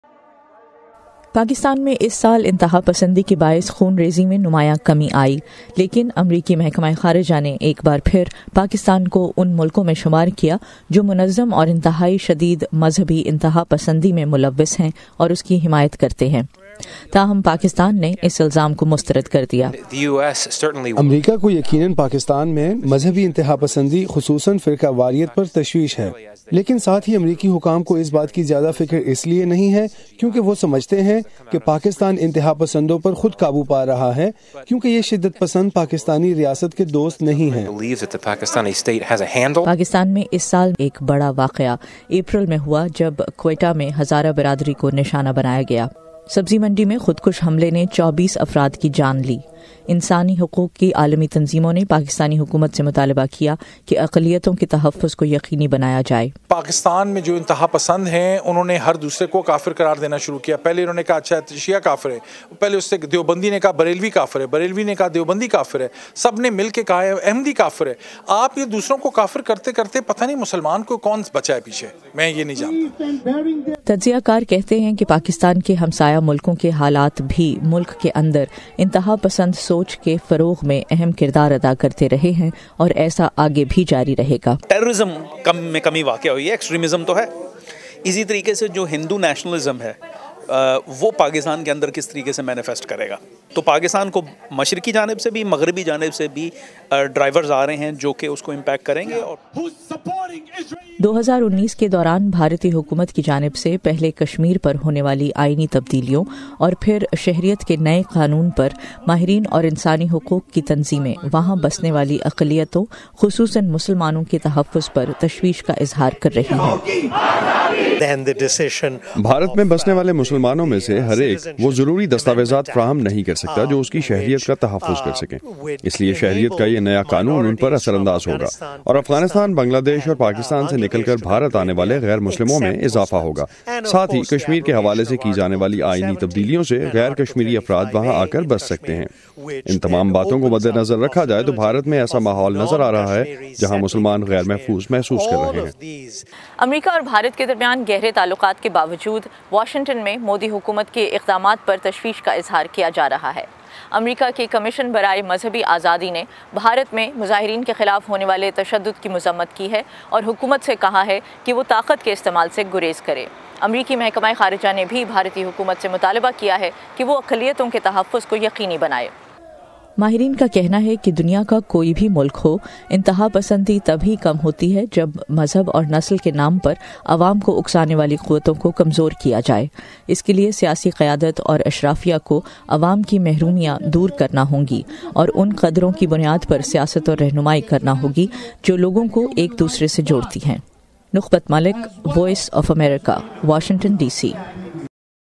Extremism high in Pakistan or in India?
پاکستان میں 2019 میں نسل اور مذہب کی بنیاد پر نشانہ بنانے کے واقعات میں کمی آئی لیکن بھارت میں حکومت کی پالیسی نے ملک کی سب سے بڑی اقلیت مسلمانوں کے مستقبل پر سوالیہ نشان کھڑا کردیا ہے۔ 2019 میں پاکستان اور بھارت میں انتہاپسندی کی صورتحال کا جائزہ نخبت ملک کی رپورٹ میں